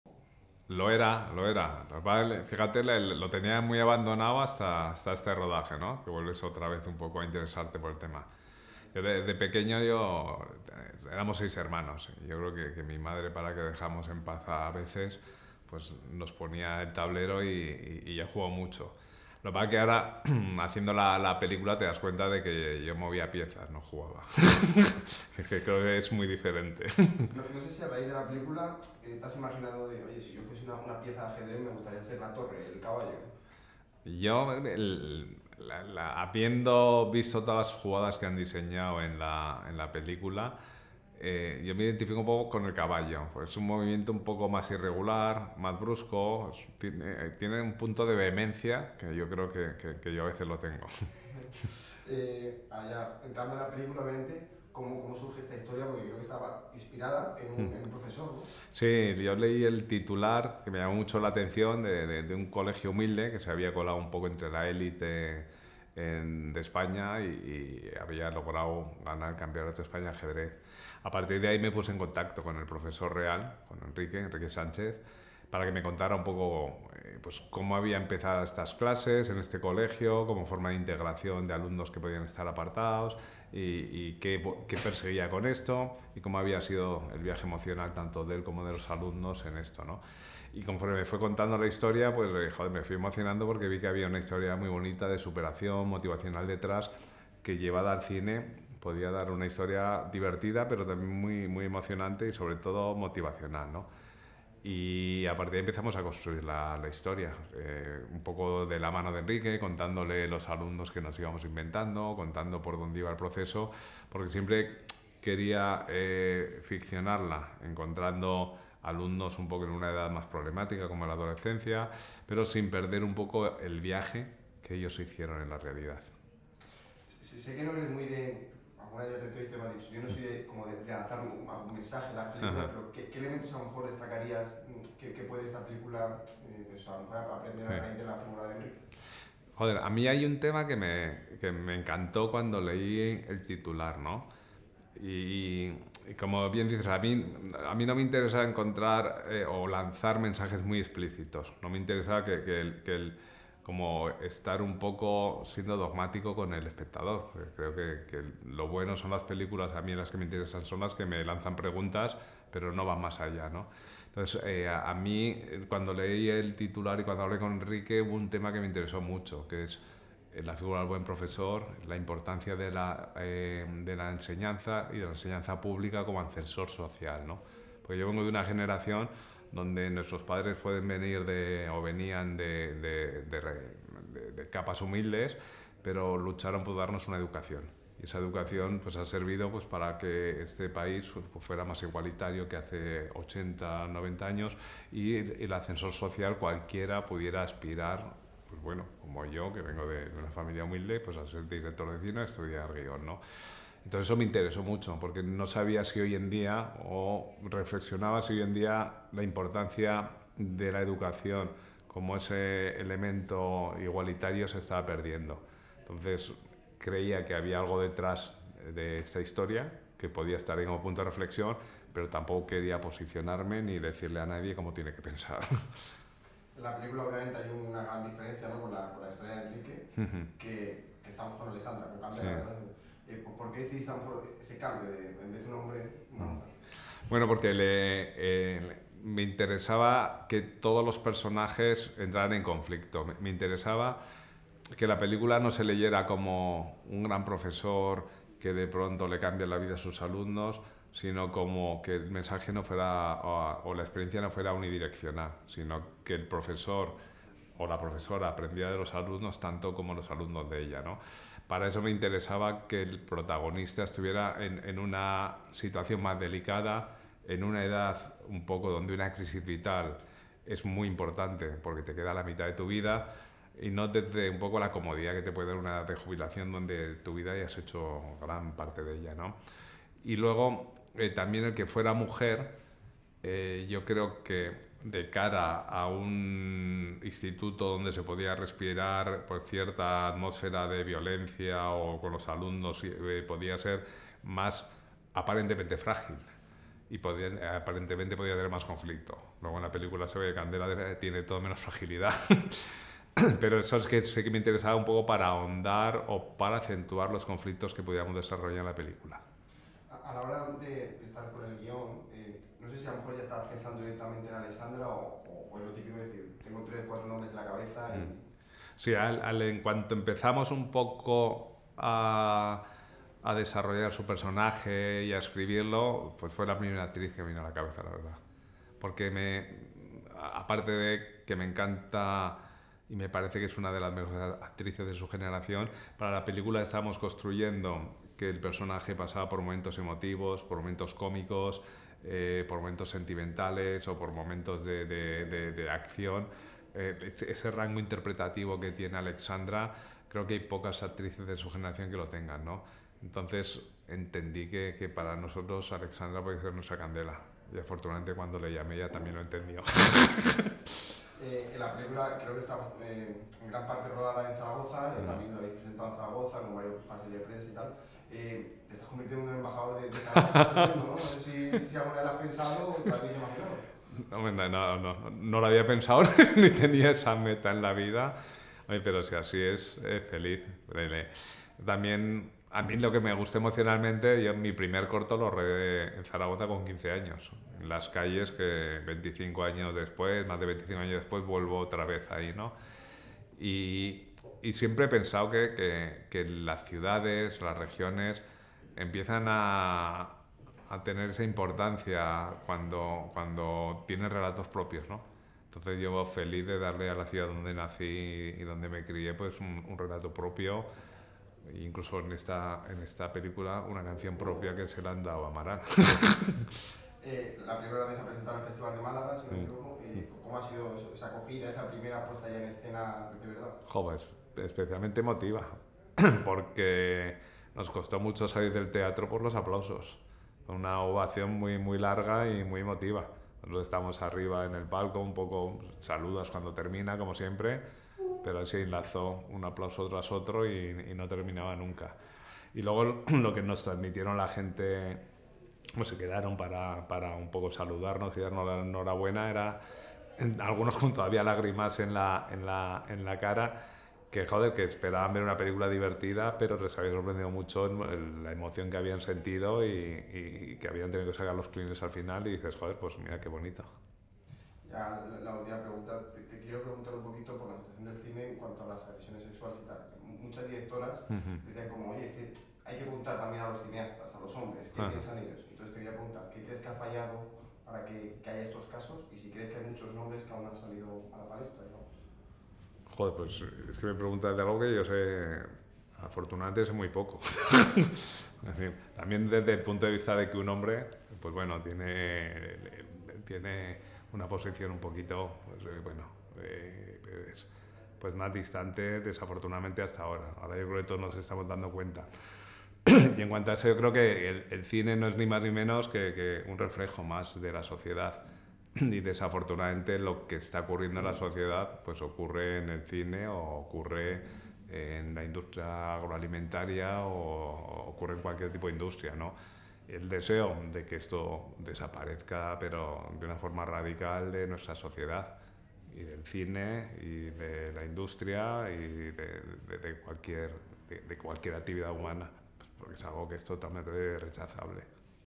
Nacho G. Velilla: "Los hombres teníamos una posición distante sobre abusos sexuales en el cine"
Declaraciones del cineasta Nacho G. Velilla, quien ha señalado que "desafortunadamente" los hombres han tenido una posición "más distante" sobre los abusos sexuales en la industria y reconoce que en la actualidad se están "dando cuenta" del problema.